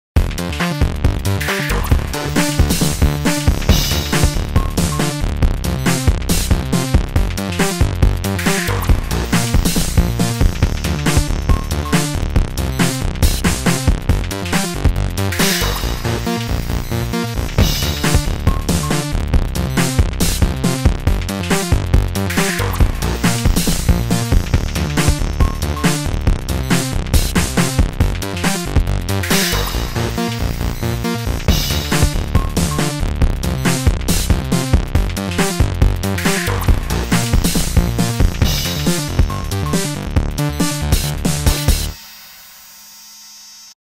Meet the band - After Effects | Videohive Projects
Here’s our latest AE template: “Meet the band”, it’s great to create a fast, modern crew presentation, movie or show title sequence or even as a logo reveal or video presentation. It has an intro and outro text scene, 10 text/photo scenes, 10 video display scenes and a logo reveal scene. You can change the color of all elements with just a few clicks. A prerendered version is included for those who don’t have Particular 2. Expressions are universal so you can use the project with any language. We hope you find it useful.

You can watch the HD preview here

Main features

Great design
Easy customization
Dynamic animation
No plugins needed
HD resolution (1280×720)
Free audio track
Universal expressions